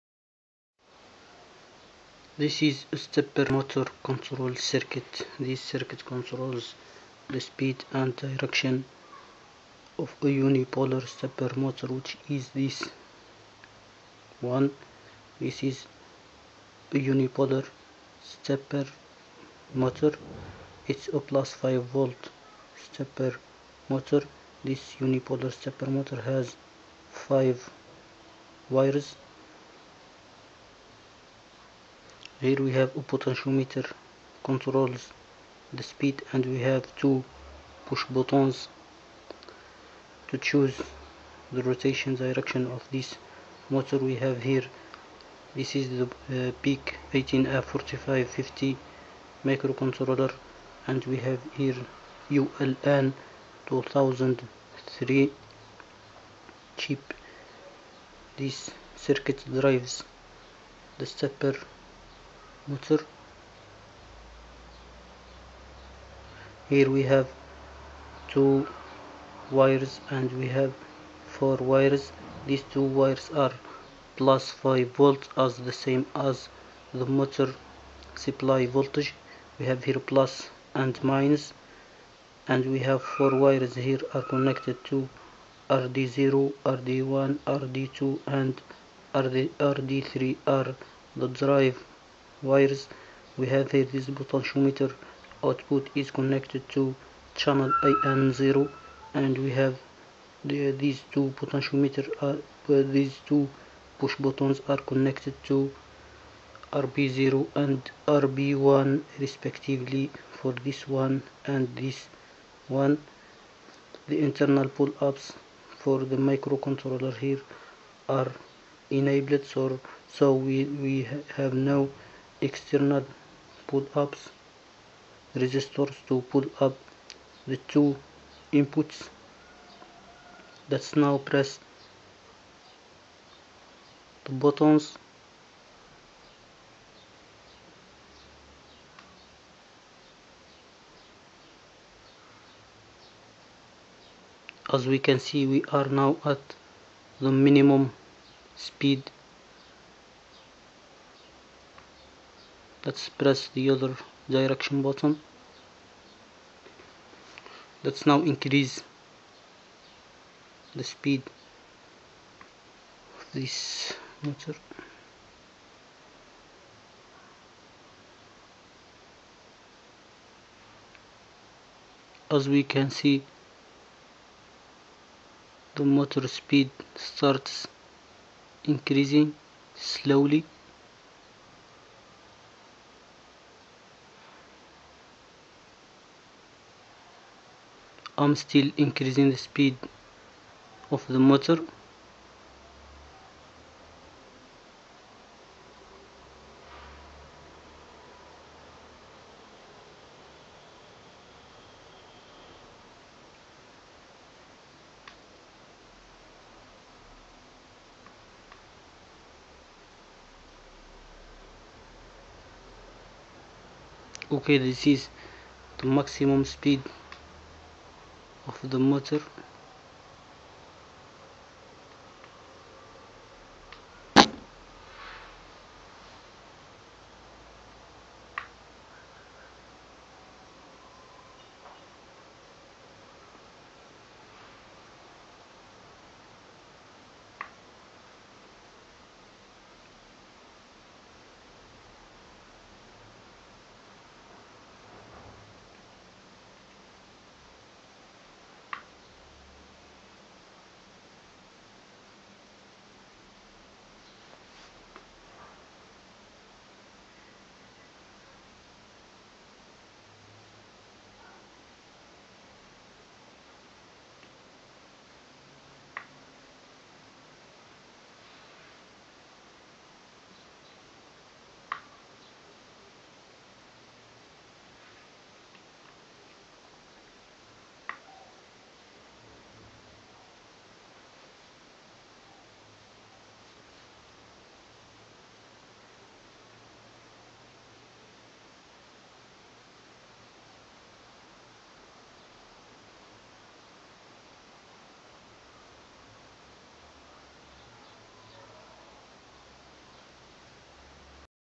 Unipolar stepper motor control using PIC18F4550 and ULN2003
Interfacing PIC18F4550 with unipolar stepper motor. Circuit schematic and CCS PIC C compiler code